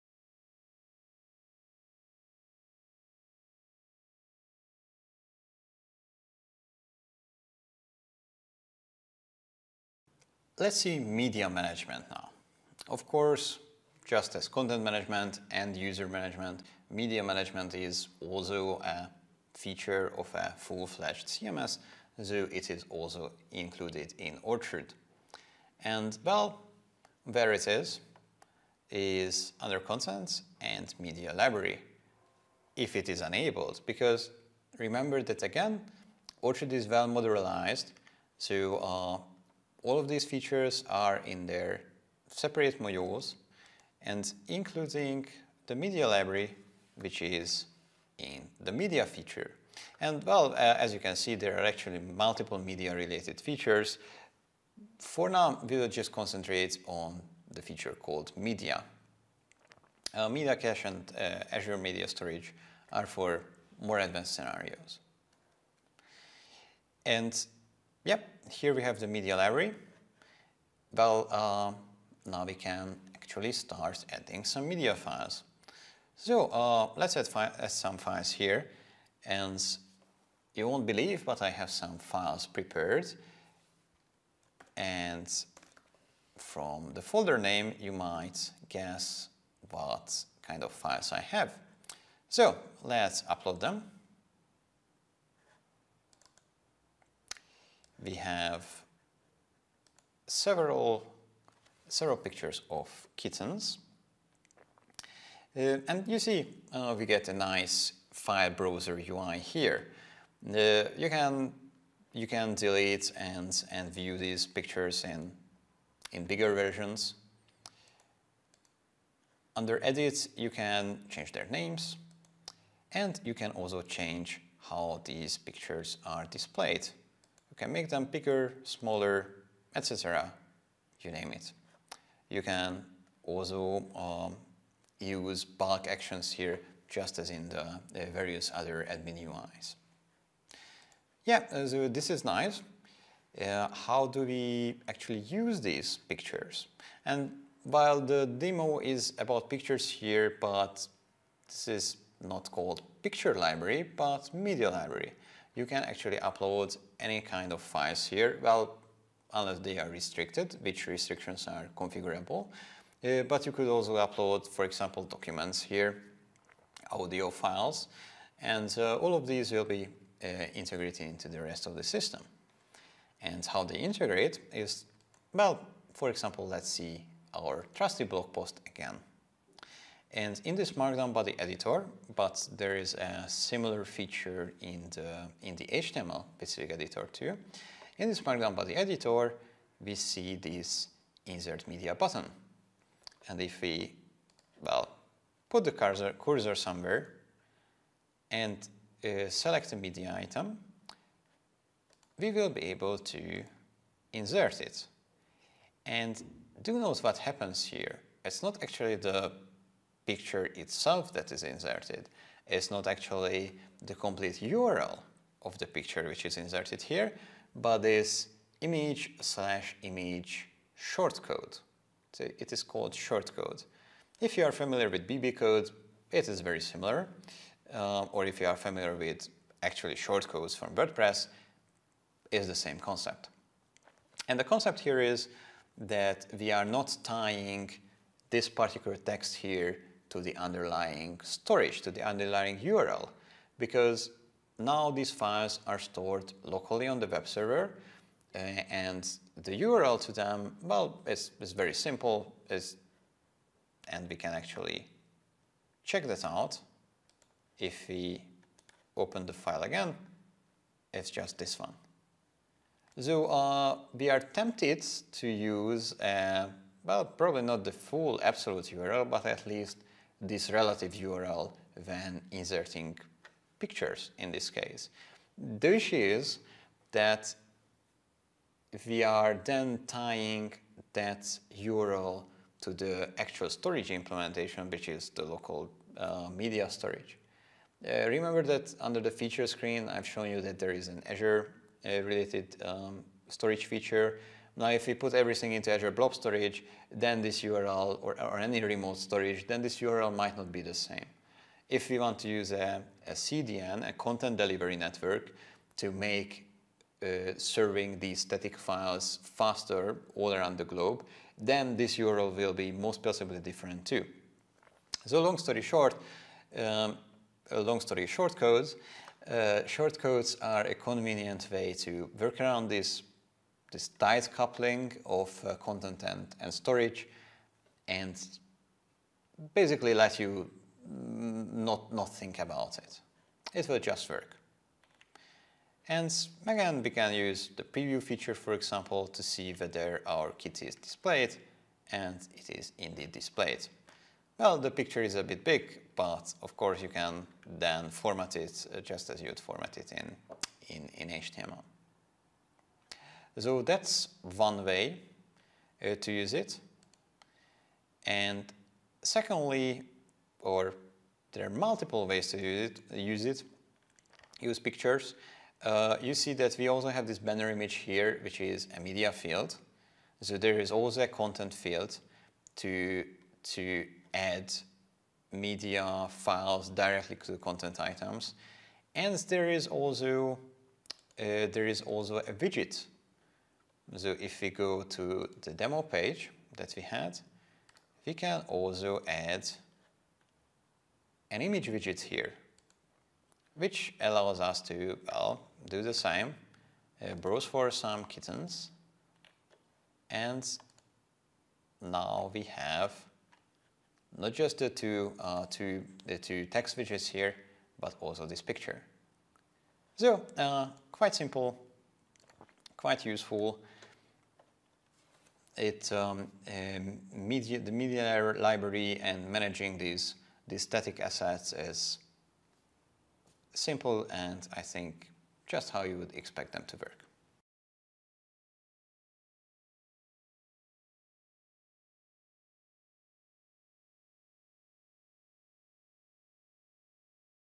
Managing media files in Orchard Core - Dojo Course 3 (10)
Dojo Course 3 - Part 10

- Media Library
- Media management
- Shortcodes

(complete solution for this course)